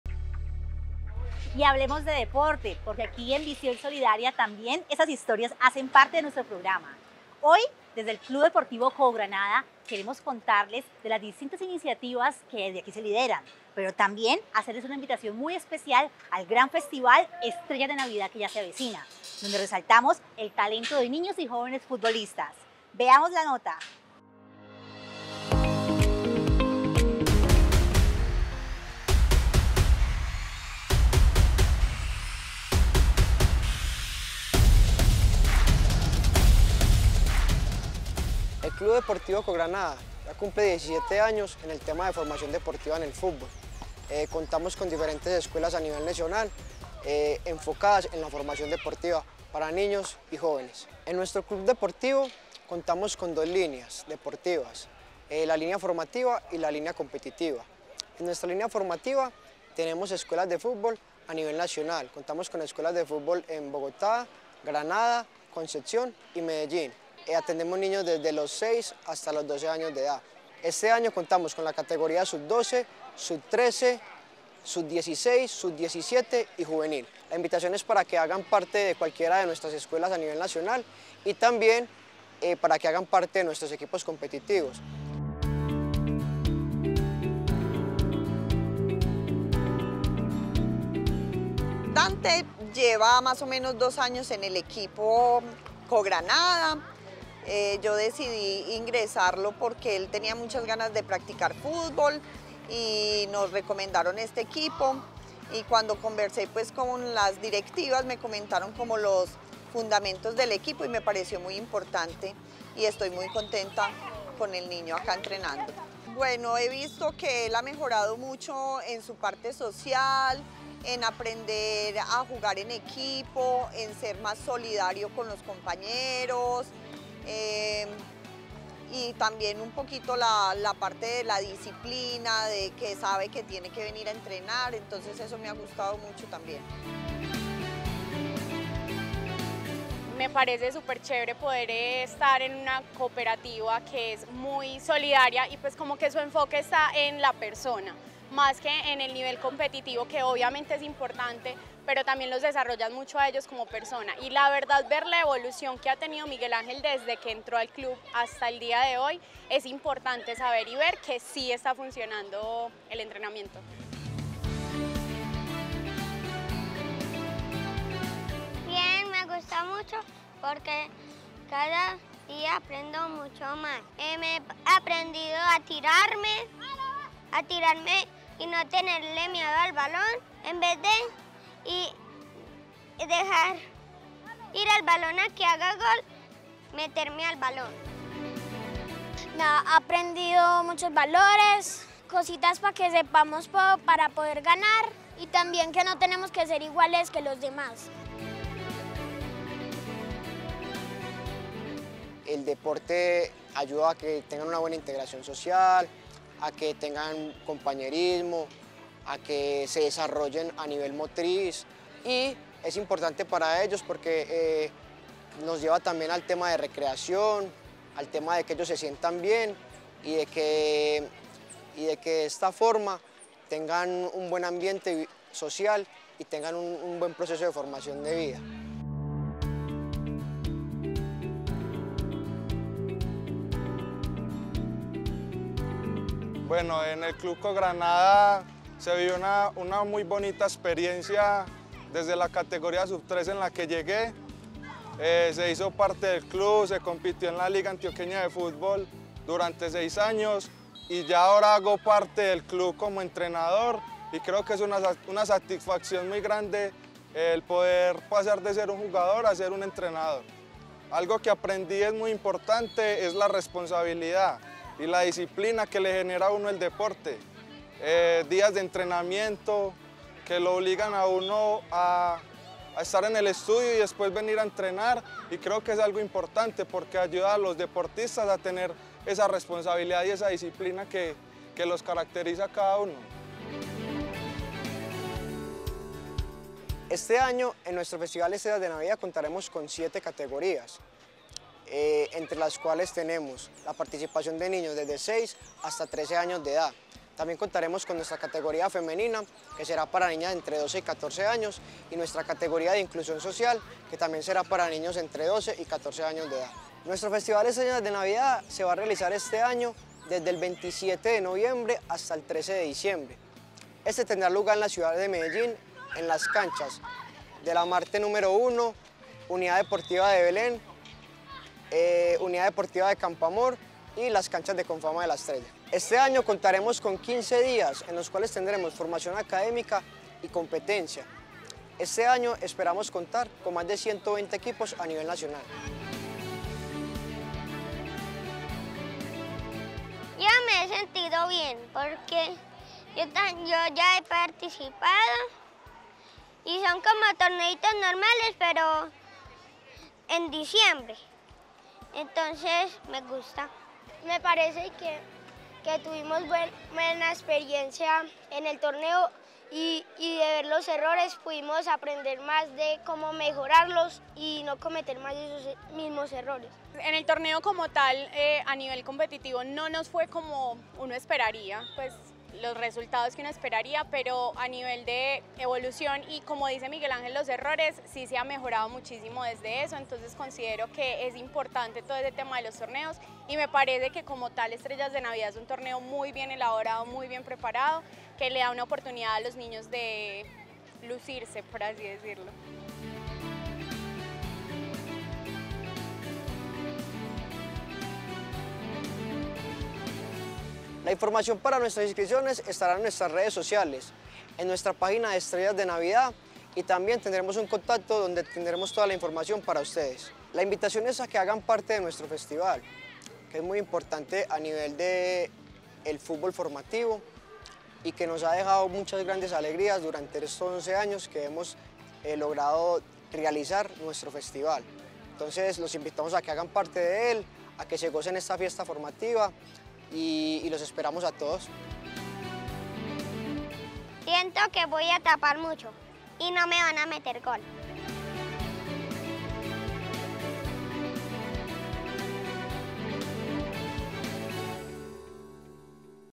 ¿Ya conoces nuestro Club Deportivo? | Visión Solidaria | Red Coogranada
Conoce nuestro Club Deportivo Coogranada y participa de nuestro torneo de Estrellas de Navidad